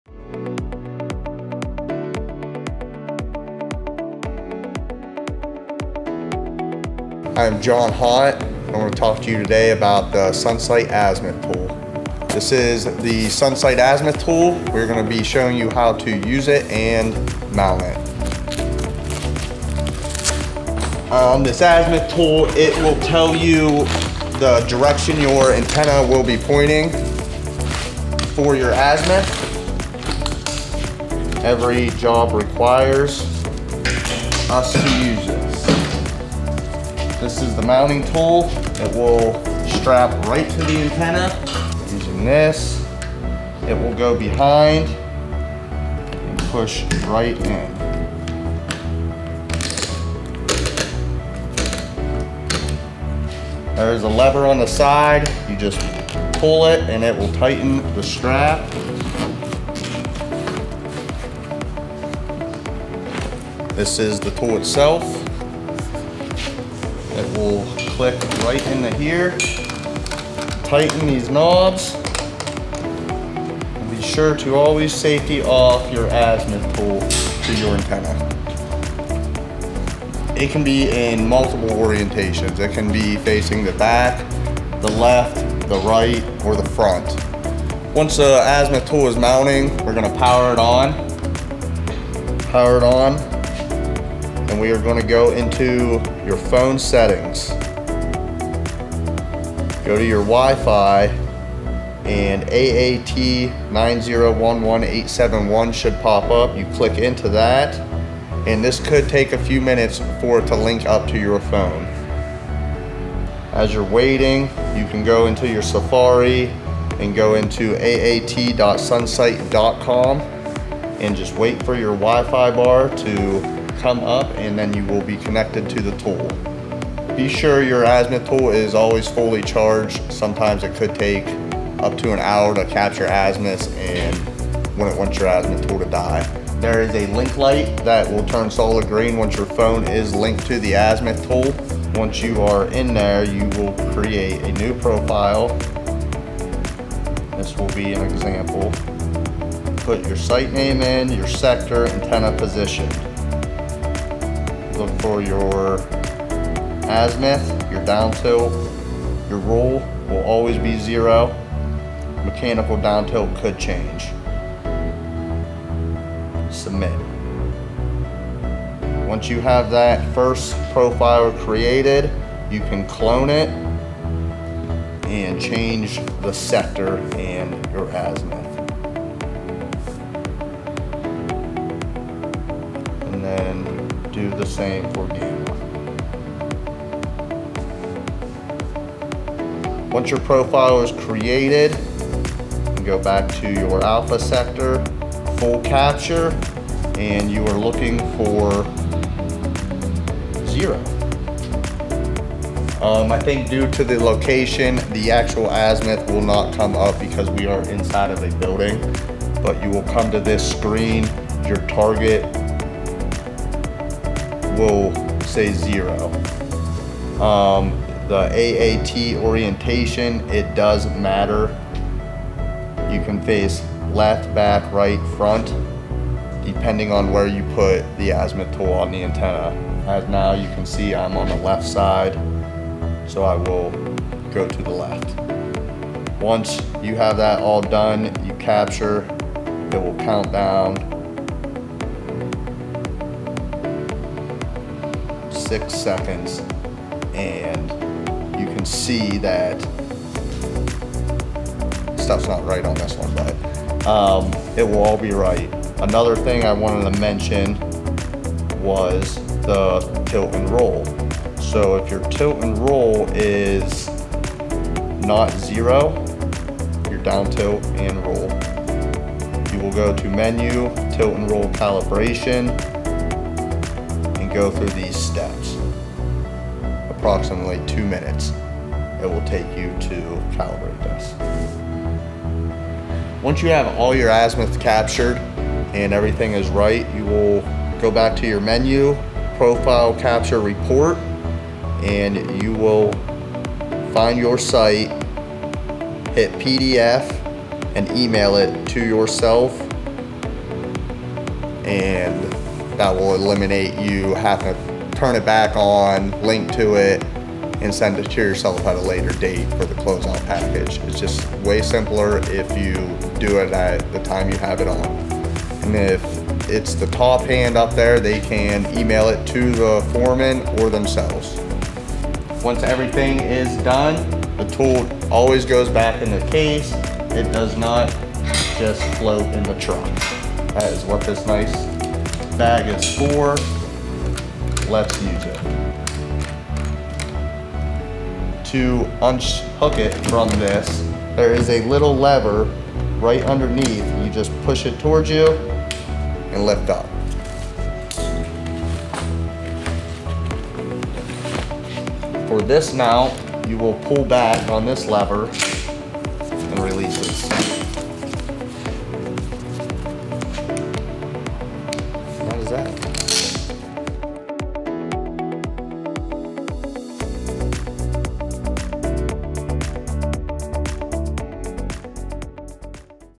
Tower Tips: Sunsight Azimuth Tool
Learn how to use the sunsight azimuth tool in this video from STG Communications Project Manager Jon Haught.